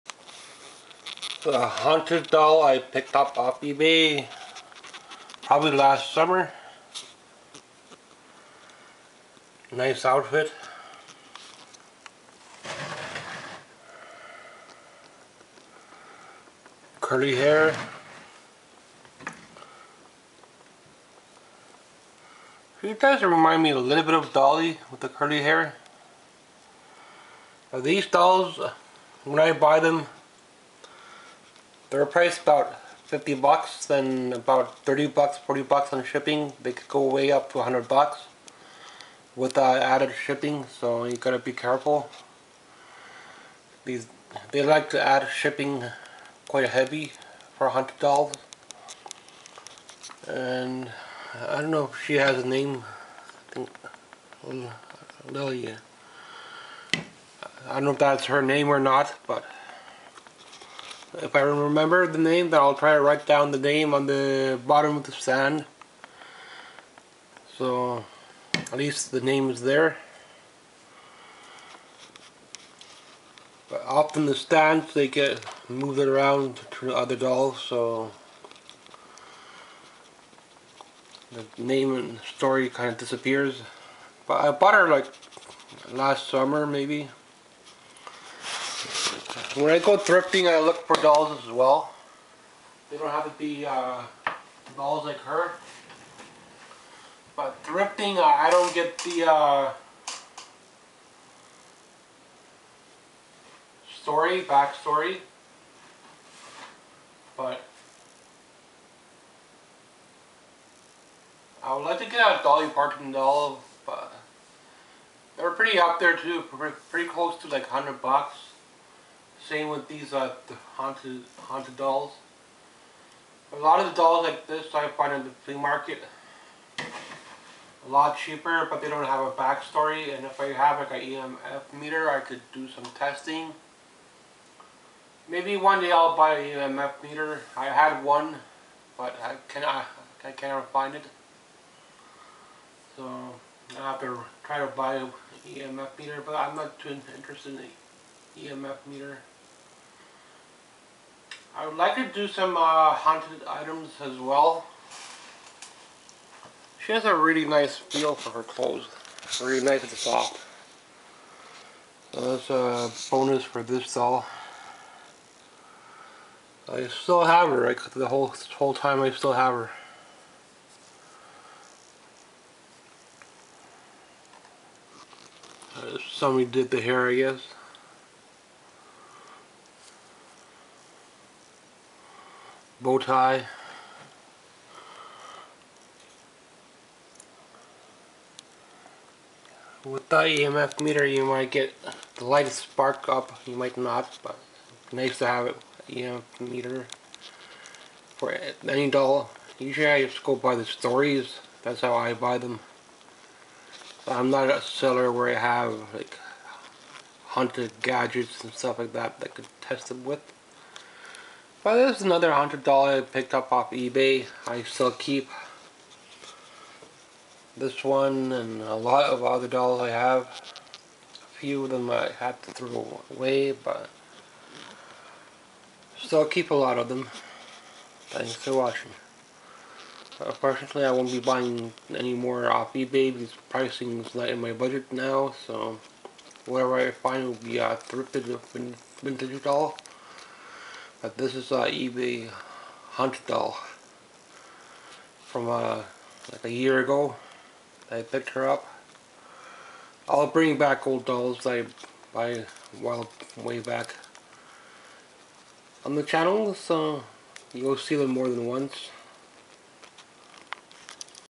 haunted doll from ebay winter
haunted doll  from ebay winter bought over a year a go ill bring some back now and then do keep lot of them